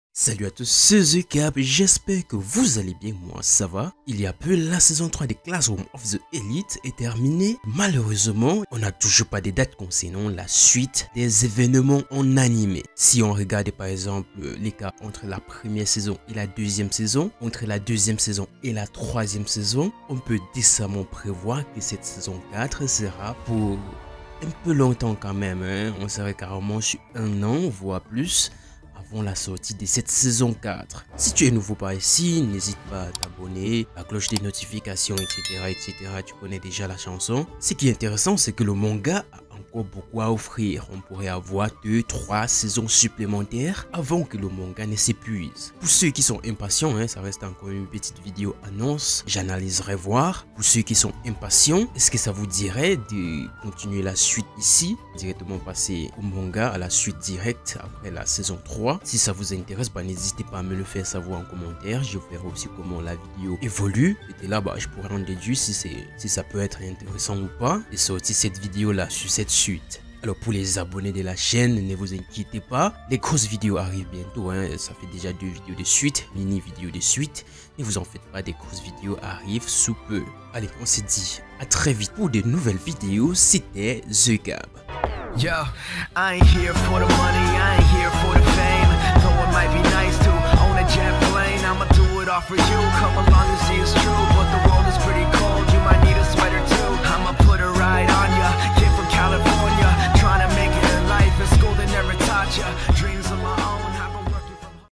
Quand et Où Regarder La SAISON 4 de Classroom of The Elite ?! Épisode 14 - SAISON 3 ?!
l'épisode 14 saison 3 de Classroom of the sera enfaite l'épisode 1 de la saison 4 !
Quand cette saison 4 ?! Où la regarder ?!

 Et si on faisait cette suite en mode review manga ?

vostfr